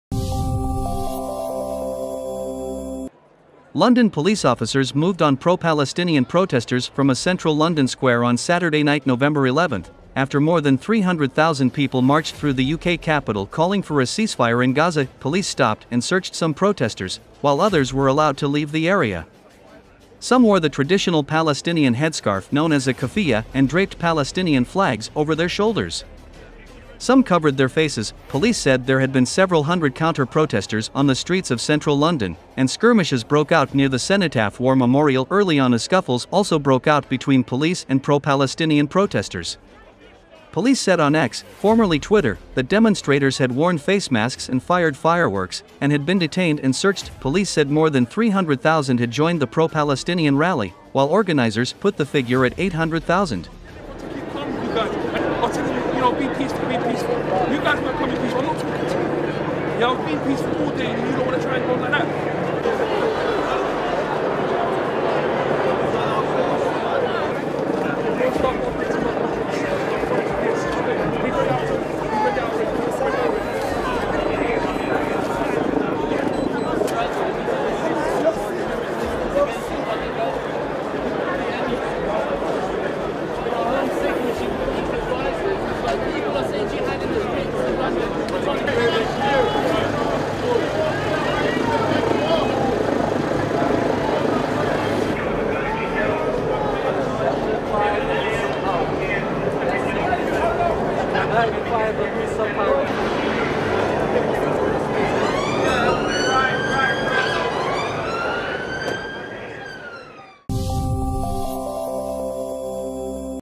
Police clear pro-Palestinian protesters from central London square
London police officers moved on pro-Palestinian protesters from a central London square on Saturday night (November 11), after more than 300,000 people marched through the UK capital calling for a ceasefire in Gaza.

Police stopped and searched some protesters, while others were allowed to leave the area. Some wore the traditional Palestinian headscarf known as a keffiyah and draped Palestinian flags over their shoulders. Some covered their faces.

Police said there had been several hundred counter-protesters on the streets of central London, and skirmishes broke out near the Cenotaph war memorial early on.

Scuffles also broke out between police and pro-Palestinian protesters. Police said on X, formerly Twitter, that demonstrators had worn face masks and fired fireworks, and had been detained and searched.

Police said more than 300,000 had joined the pro-Palestinian rally, while organisers put the figure at 800,000.

Tensions had been running high before Saturday's march - the biggest in a series to show support for the Palestinians and call for a ceasefire in the Gaza Strip - after interior minister Suella Braverman called them "hate marches" led by "mobs".